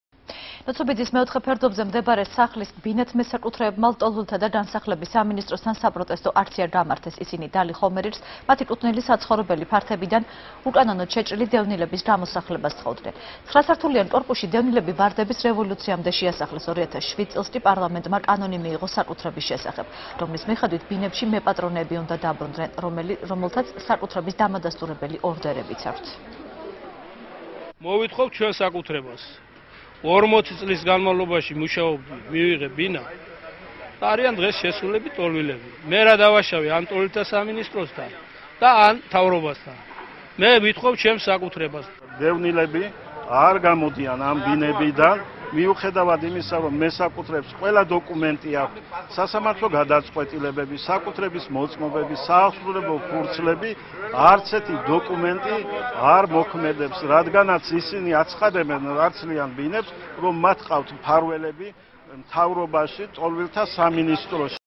ახალი 6 | ბინათმესაკუთრ. პროტესტი | 10.07.12
ნუცუბიძის მეოთხე ფერდობზე მდებარე სახლის ბინათმესაკუთრეებმა ლტოლვილთა და განსახლების სამინისტროსთან საპროტესტო აქცია გამართეს.  ისინი დალი ხომერიკს მათი კუთვნილი საცხოვრებელი ფართებიდან უკანონოდ შეჭრილი დევნილების გამოსახლებას სთხოვდნენ.  9 სართულიან კორპუსში დევნილები ვარდების რევოლუციამდე შეასახლეს, 2007 წელს კი პარლამენტმა კანონი მიიღო საკუთრების შესახებ, რომლის მიხედვით, ბინებში მეპატრონეები უნდა დაბრუნდნენ, რომელთაც საკუთრების დამადასტურებელი ორდერებიც აქვთ.